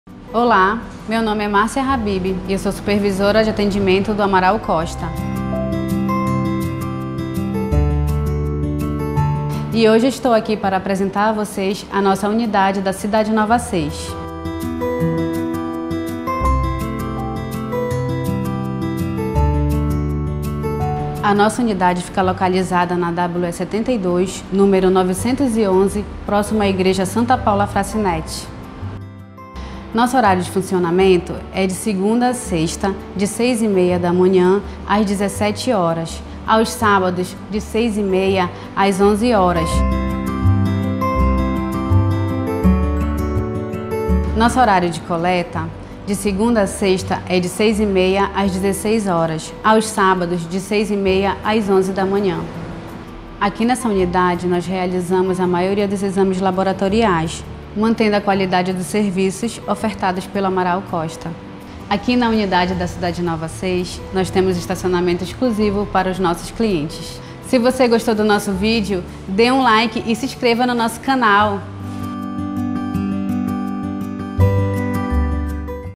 CONFIRA A UNIDADE DA CIDADE NOVA VI
A Unidade Amaral Costa da Cidade Nova VI fica na WE 72 nº 911, próxima a Paróquia Santa Paula Frassinetti. Lá é possível realizar seus exames laboratoriais com conforto e rapidez.